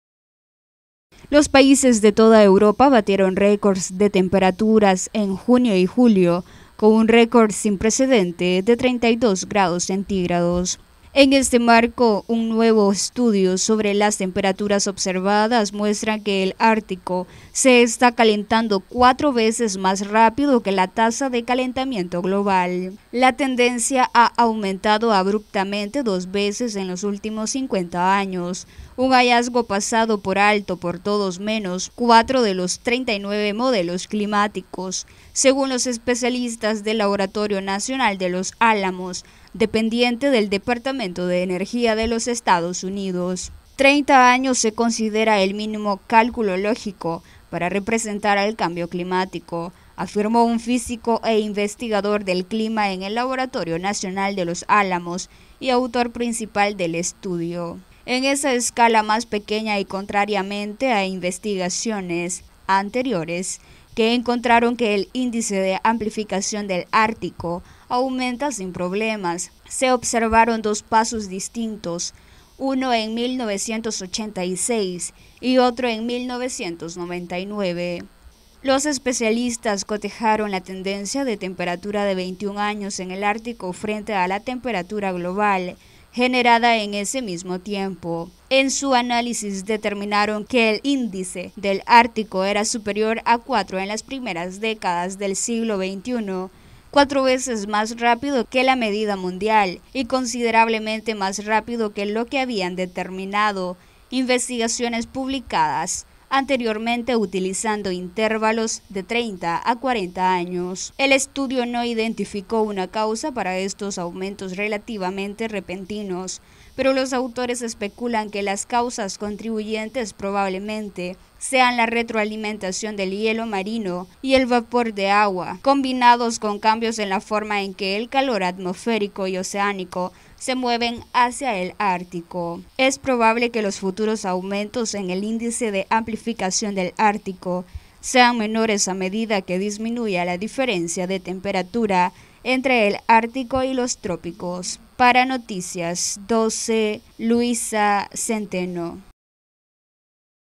La temperatura del ártico aumenta cuatro veces más rápido que el promedio global
Interncionales La temperatura del ártico aumenta cuatro veces más rápido que el promedio global, según nuevo informe de científicos